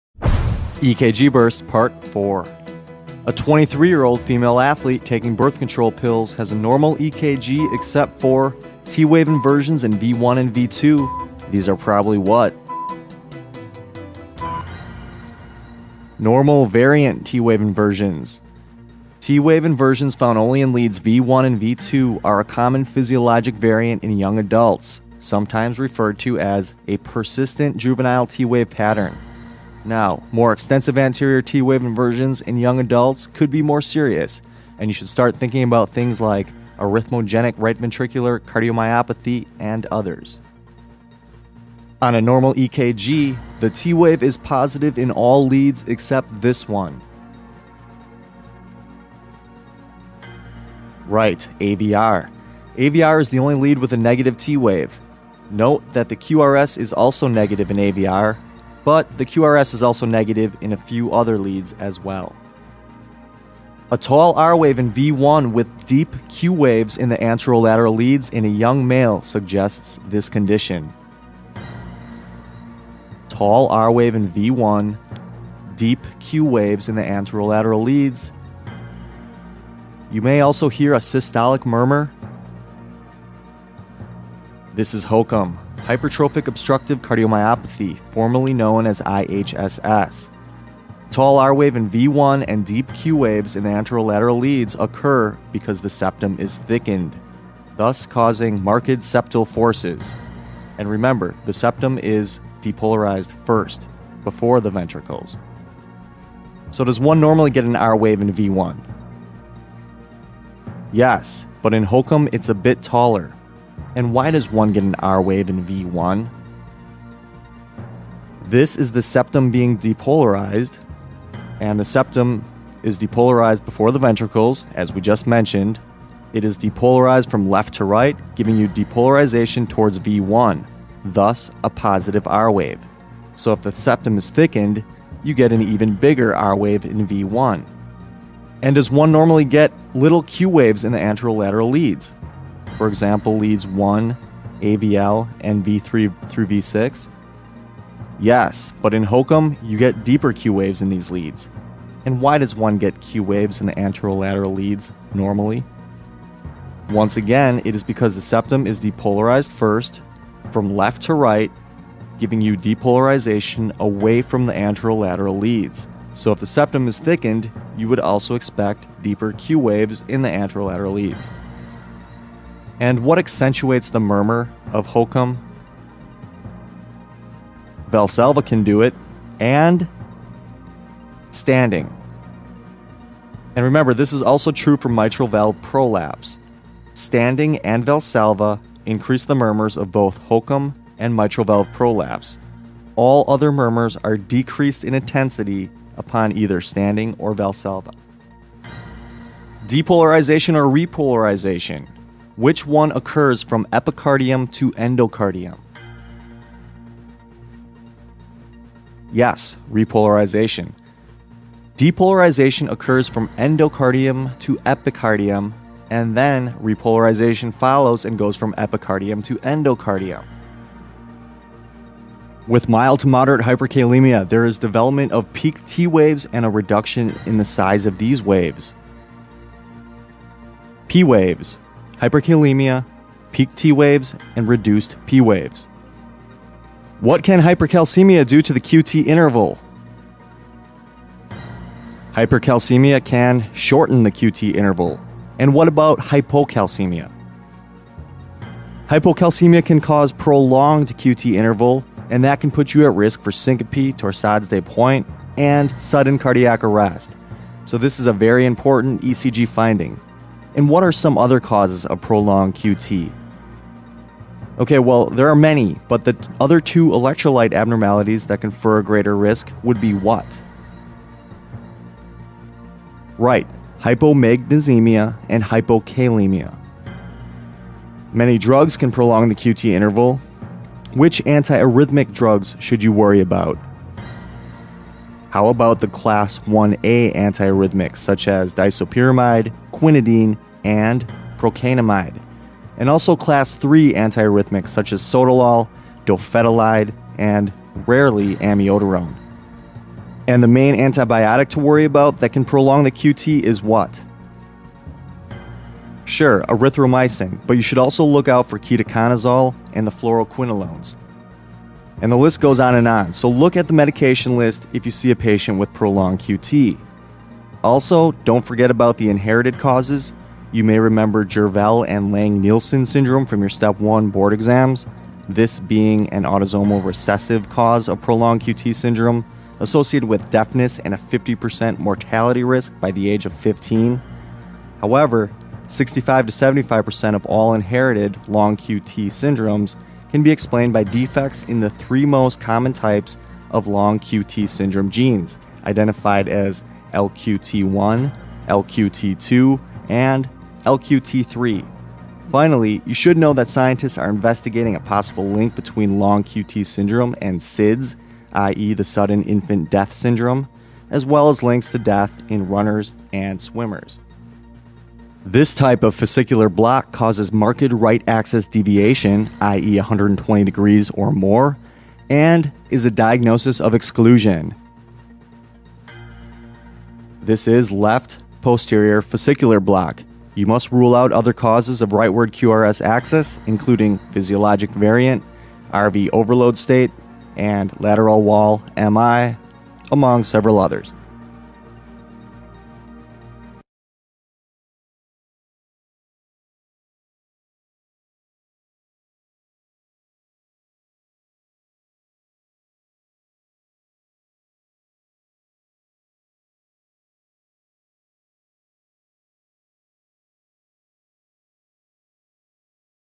ECG Q&A Part 4
persistent juvenile T wave pattern, hypertrophic obstructive cardiomyopathy (HOCM),  hyperkalemia, left posterior fascicular block (left posterior hemiblock), prolonged QT interval